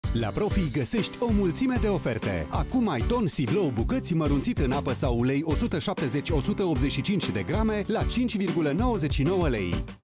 La Profi găsești o mulțime de oferte! Acum ai Ton Siblou 170/185g la un pret special.
La Profi găsești o mulțime de oferte! Acum ai Ton Siblou bucăți, mărunțit în apă sau ulei 170/185g la un pret special. Oferta supusa unor termene si conditii. Profi. Zilnic preturi mici.